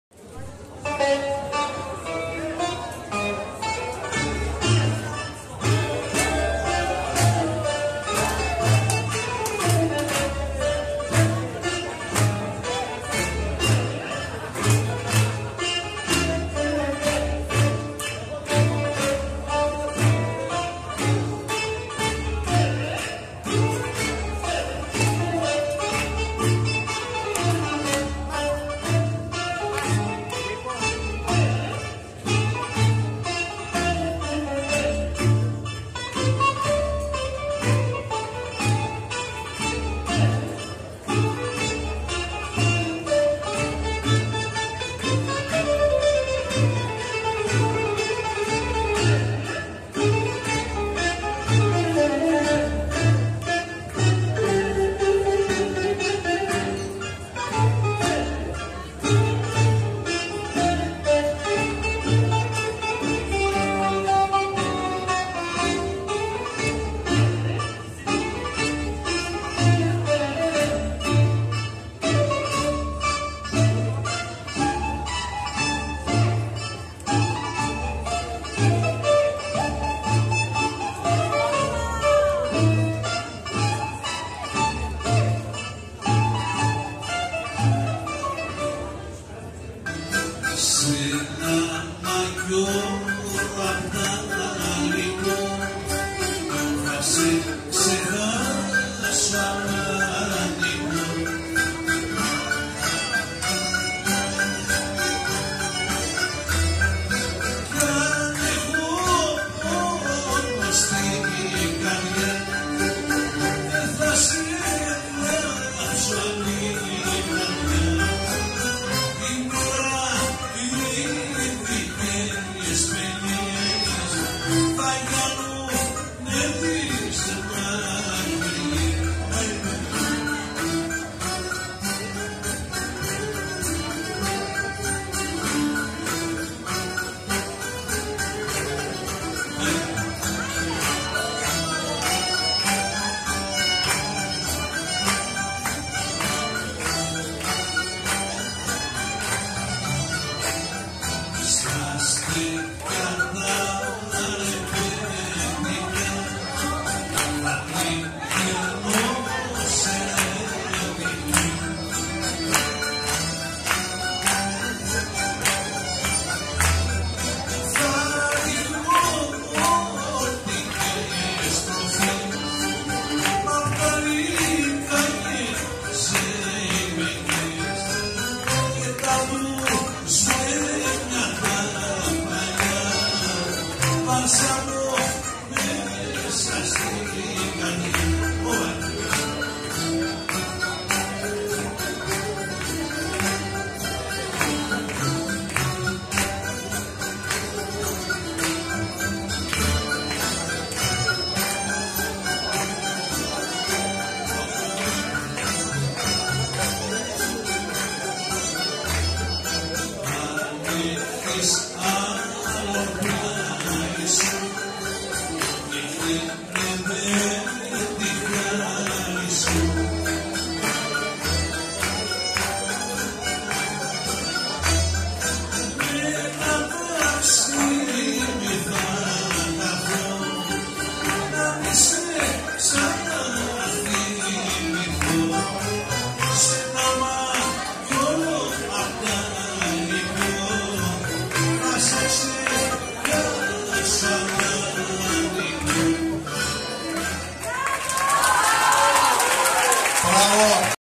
Ζεϊμπέκικο(Παλιό) & Απτάλικος-zeibekiko Panagiotis Katsikis "zeus"!
Xορεύει ο Παναγιώτης Κατσίκης στην Κων/πολη και στο 7ο συνέδριο ελληνικού χορού 2023 Kaan Temizel -Kakapedia